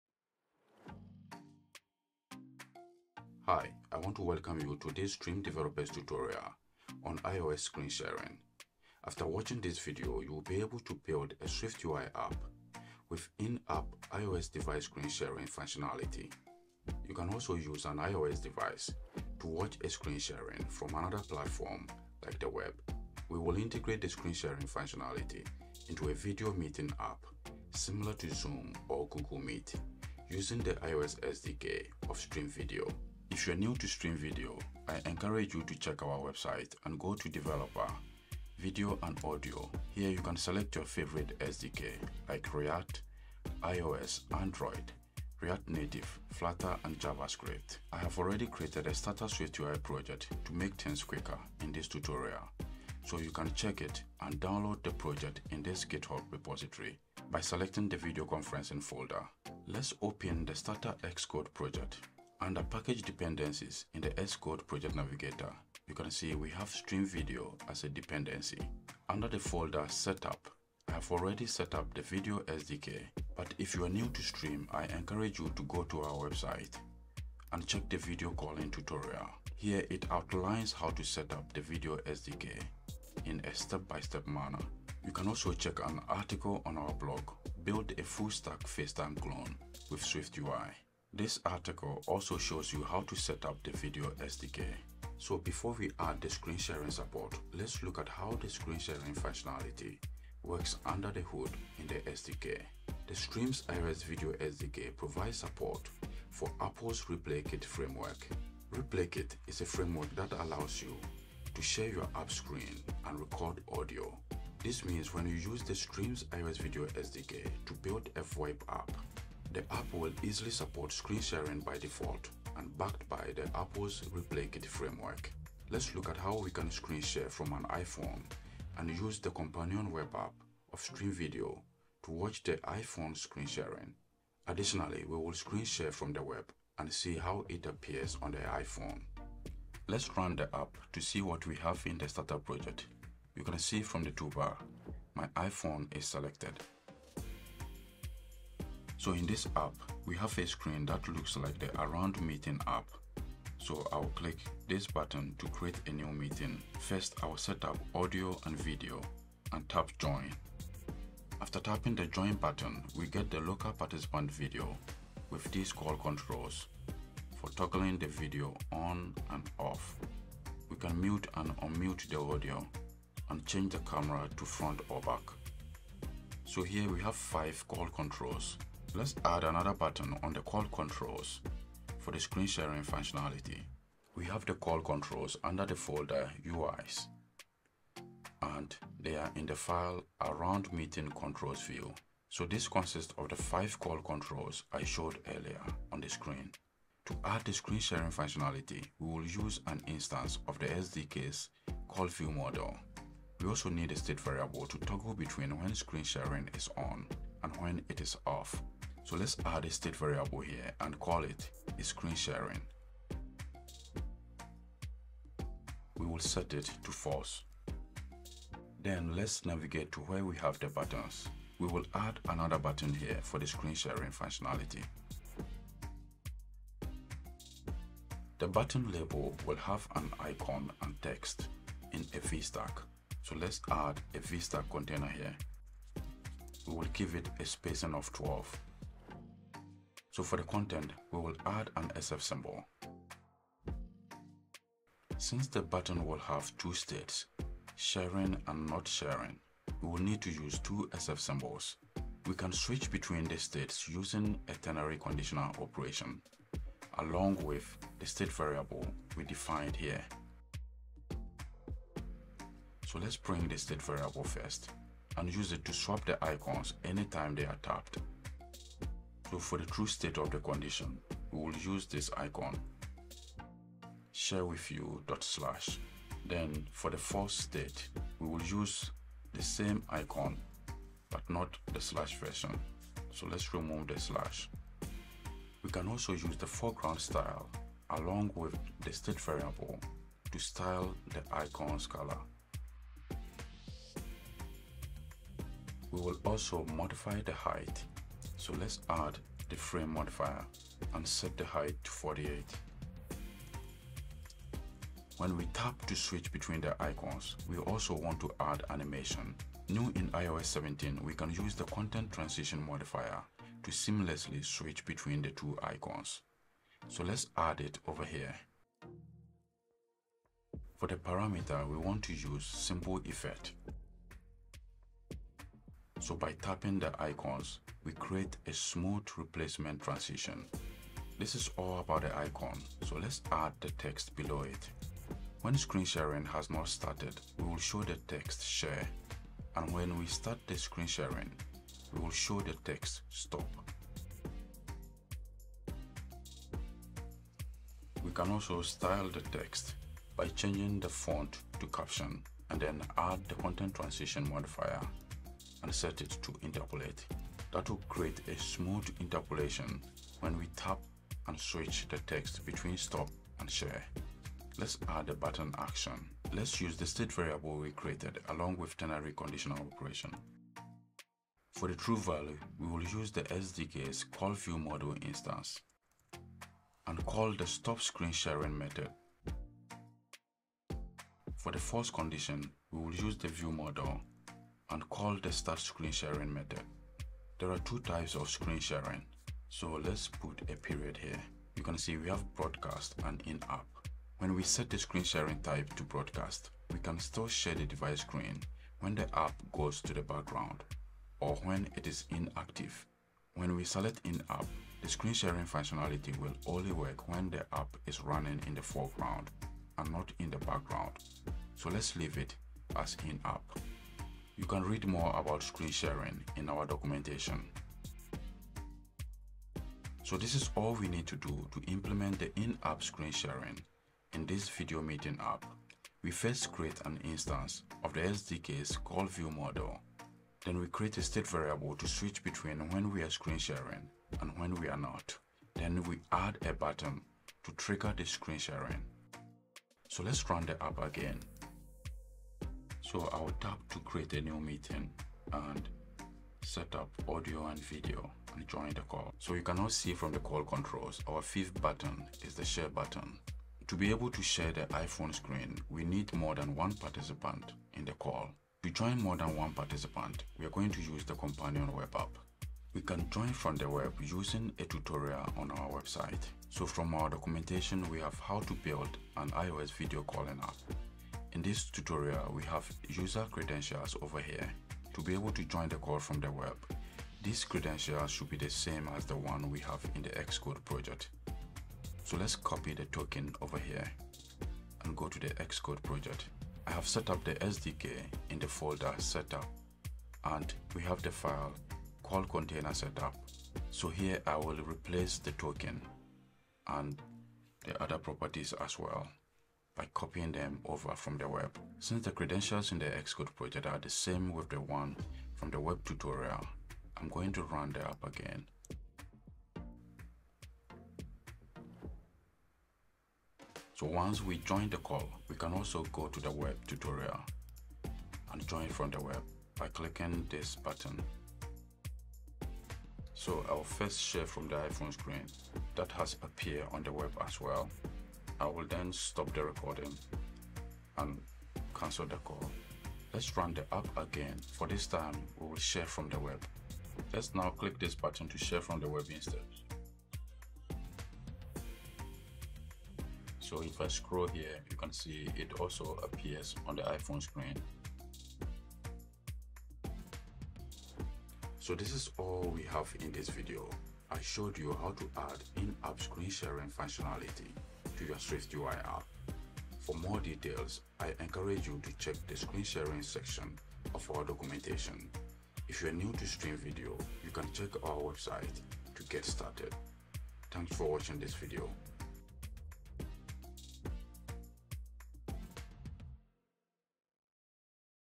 Using SwiftUI and ReplayKit: Build an iOS Screen-Sharing App
Discover how to build an iOS in-app screen sharing to capture your app’s screen, audio, and mic content. Learn to integrate Stream’s iOS Video SDK with your SwiftUI app gaplessly to create a tremendous screen-sharing experience.

Timestamp
00:00 Introduction
02:07 Tutorial Chapters
02:29 The Final Project
03:26 Add Screen-Sharing Support
06:58 Visit the Screen-Sharing Docs
08:44 Screen-Share the iPhone's Screen
08:57 Screen-Share From the Web
09:17 Wrap-Up